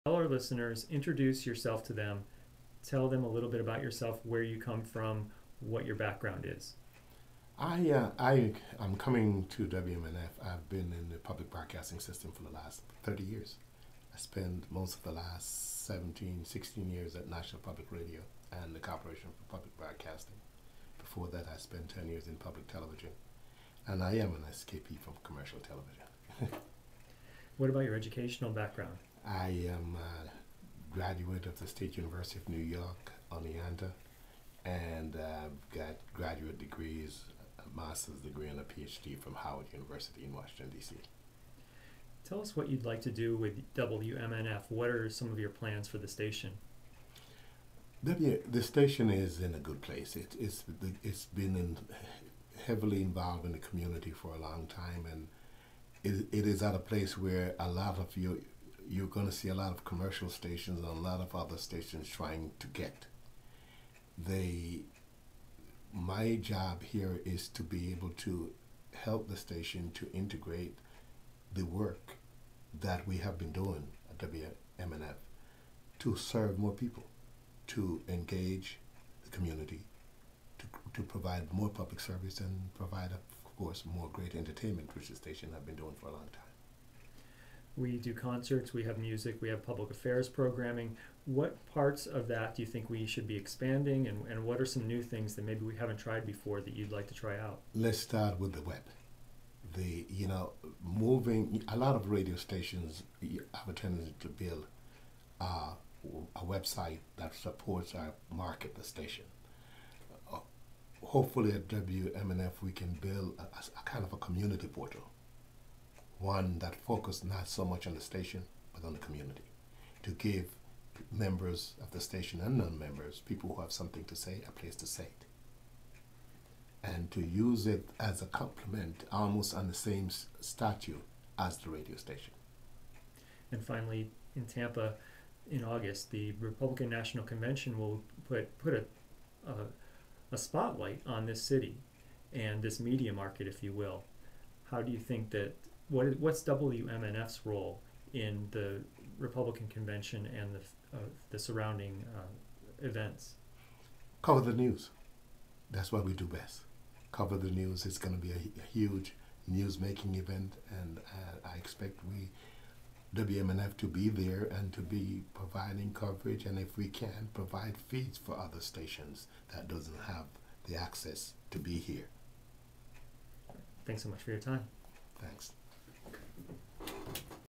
New WMNF station manager Sydney White: WMNF News
WMNF News interview with new WMNF station manager Sydney White about his background and the future of 88.5 FM Tampa.